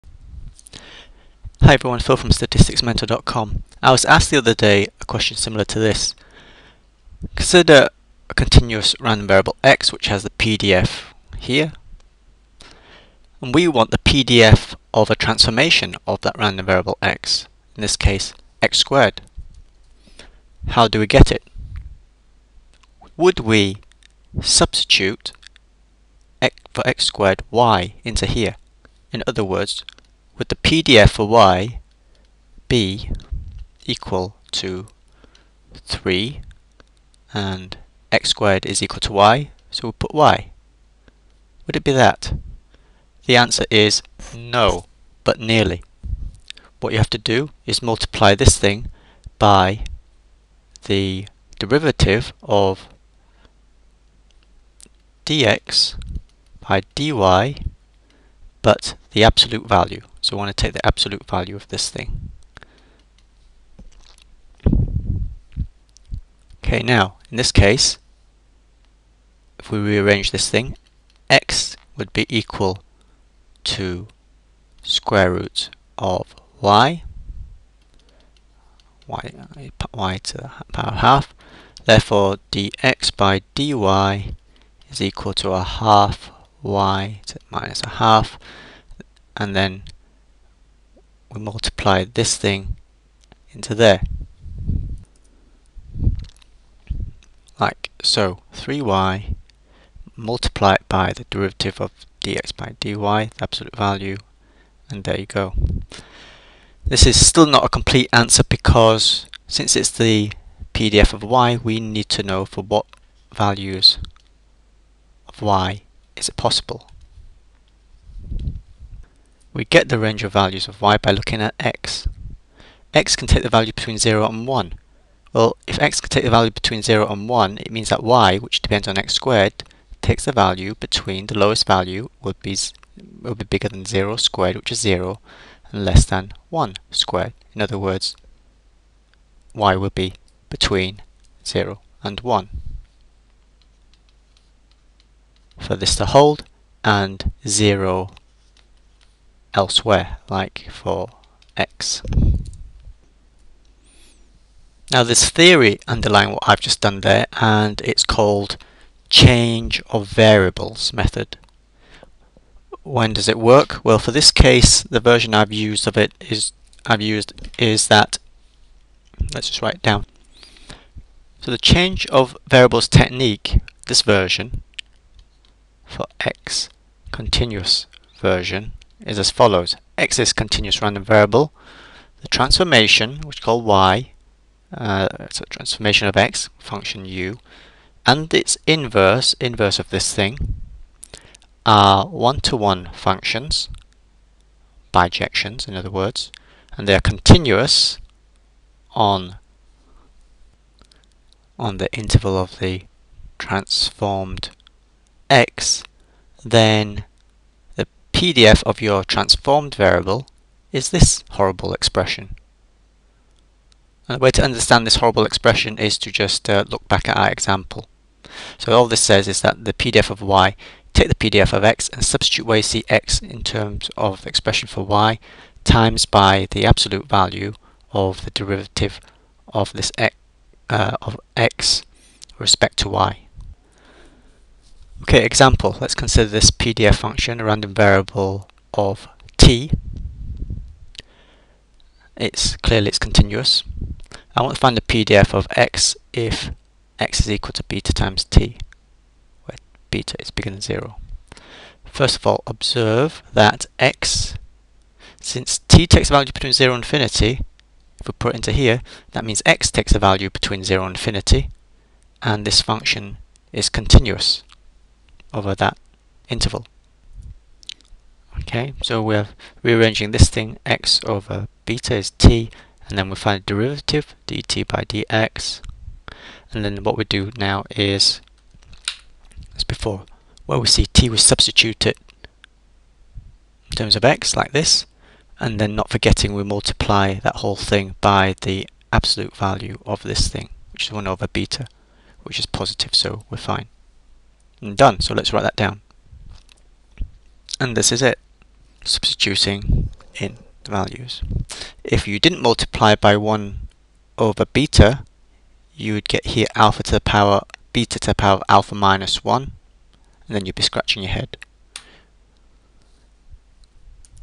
#13 Probability density function of a monotone transformed variable using change of variable method
Given a random variable X - to get the pdf of a transformed variable, Y, do we just substitute X in terms of Y? Not quite. We can use the Change of variable method to find the new pdf for a transformed X given X.
NOTE: Version presented for continuous X where the transformation g on Y=g(X) is monotone ie one to one correspondence.